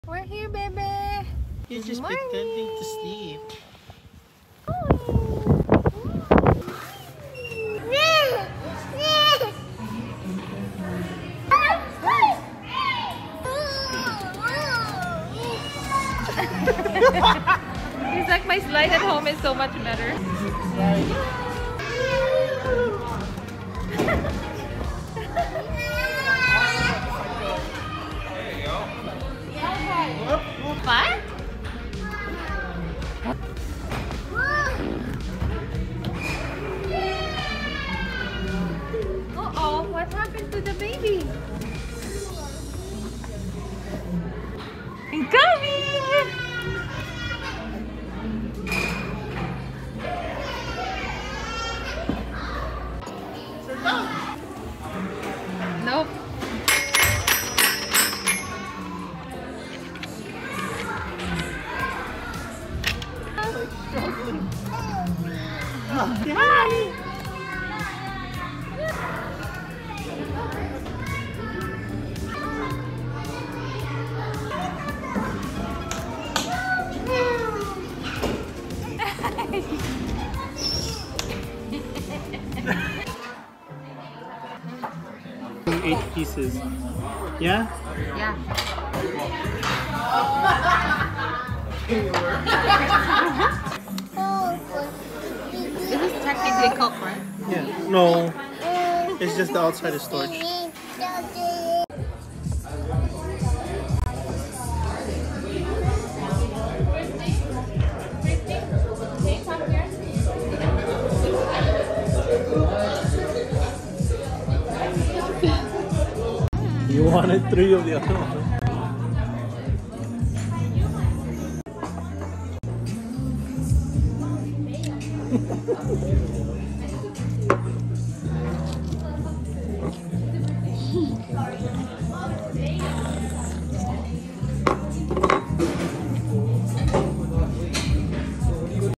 Geo Journals: This Indoor Playground is Every Toddler’s Dream!😍
Join us as we explore an indoor playground perfect for toddlers and families! With plenty of space to climb, slide, and play, it’s a great spot for fun no matter the weather. Watch our toddler dive into the adventure and see why this place is a must-visit for family adventures!

📍 more fun with bounce
📍August 8